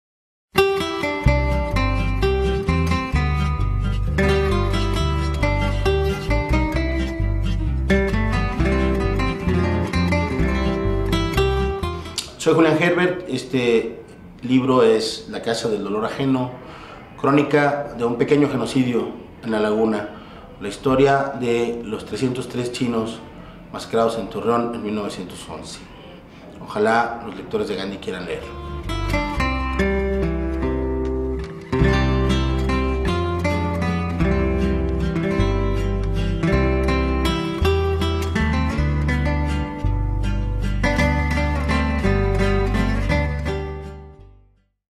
Julián Herbert se vende con "La casa del dolor ajeno"
Le dijimos a Julián Herbert: “Tienes unos minutos para pensar en un comercial sobre tu propio libro”. Después grabamos el anuncio que ahora les mostramos. Esto es lo que se obtiene cuando un "Escritor se vende"…